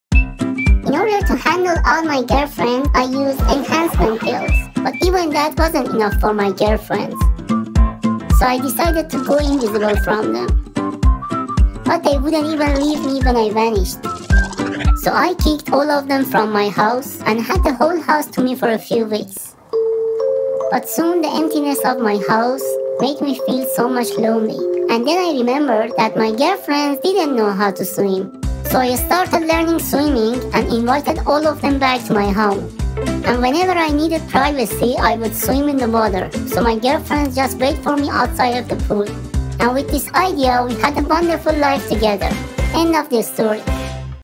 Cute Robot Handling too many Girlfriends ( Unreal Engine gameplay captured )
I'm a cute little robot in my game world where I have just too many girlfriends to handle & these are my struggles 😋🤣🤖